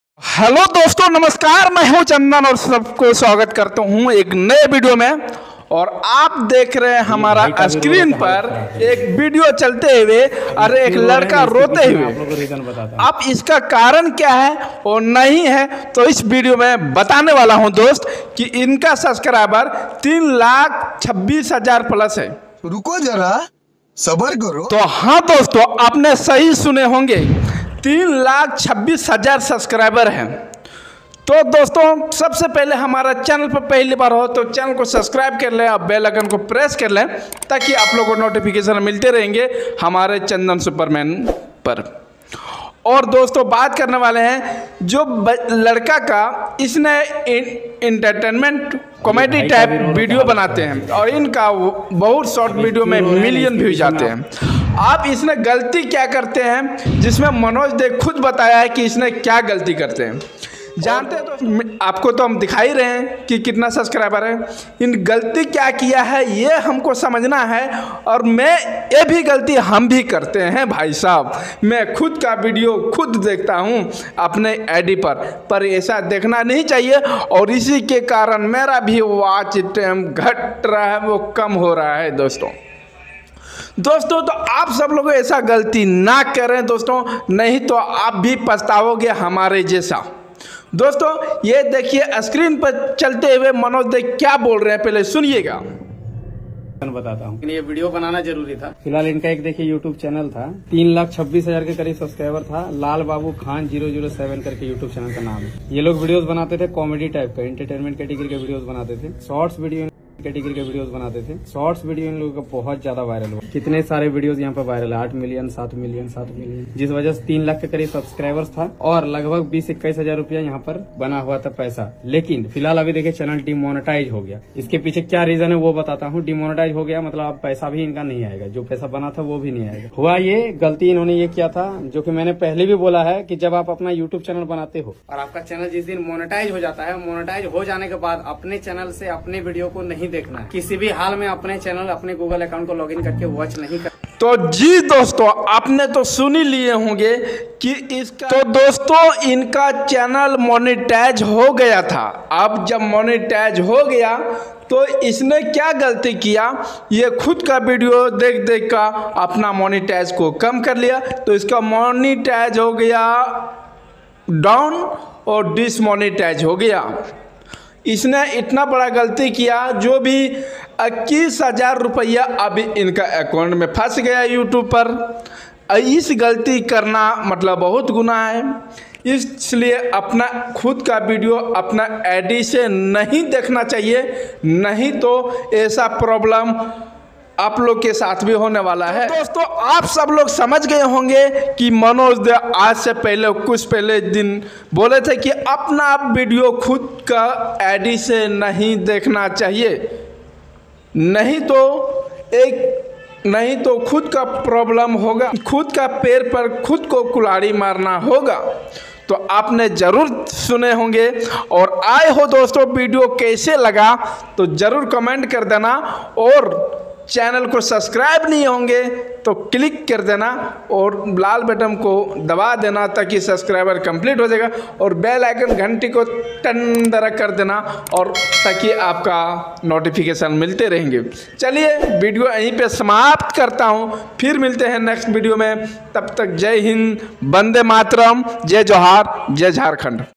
How to monetize//Monetize Hone ke Bad kya Galati Karna Chahiye//who is Manoj Day Jharkhand 2023!!
Hello friends kaise ho aap sab log ummid karta hu Sahi honge Namaskar main Chandan sabka bhai Aaj ka video bahut hi khas hai

doston aise Galti aap log na Karen